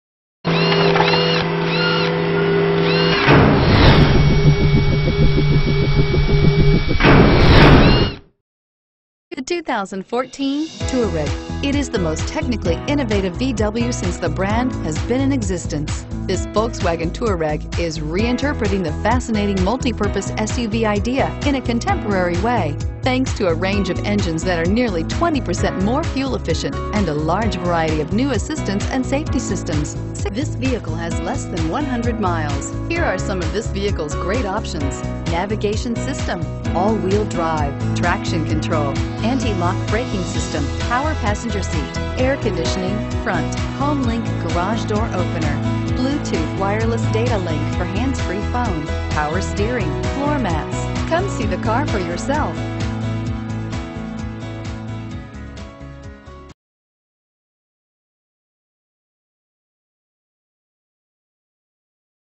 2014 Volkswagen Touareg Garden Grove CA ED009293
2014 Volkswagen Touareg

Don't miss this 2014 Volkswagen Touareg. It's equipped with Automatic transmission and features a GRAY exterior. With 12 miles, You'll want to take this car home. Make a great choice today.